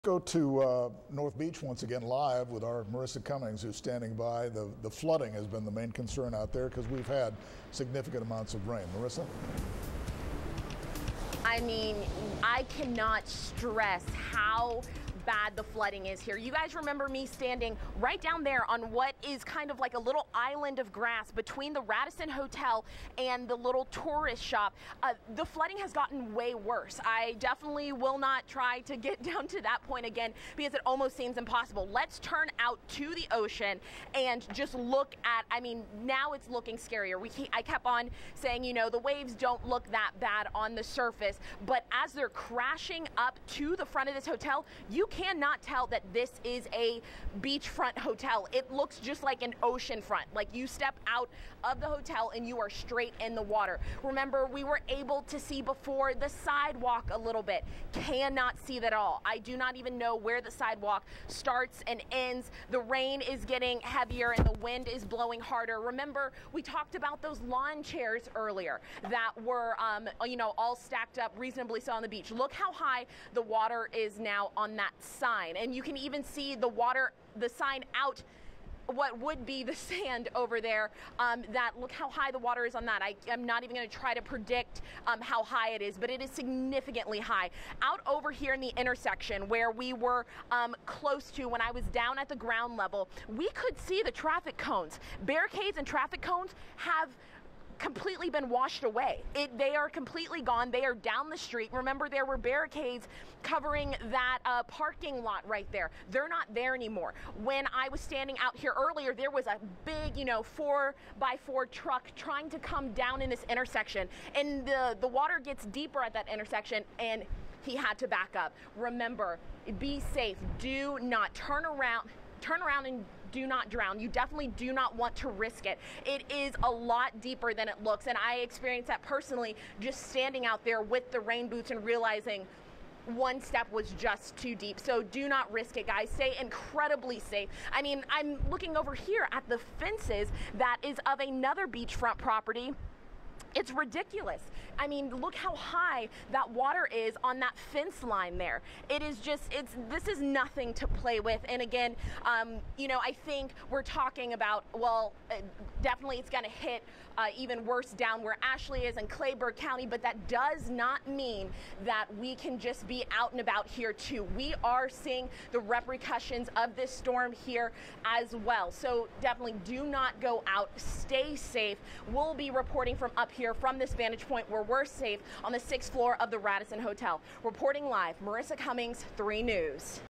Conditions deteriorating in North Beach
Flooding has gotten much worse on North Beach.